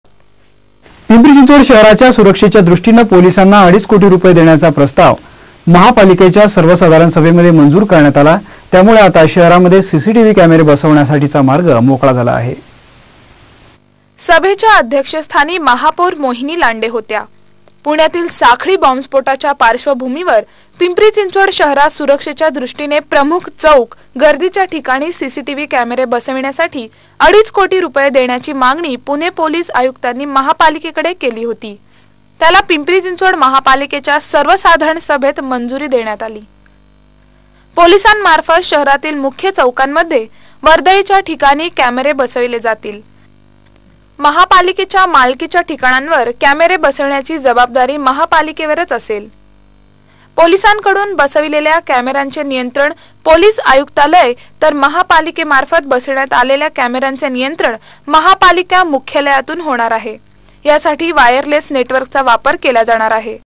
पिंपरी CCTV प्रस्ताव.
CCTV कॅमेरे खरेदीसाठी अडीच कोटींची मंजुरी.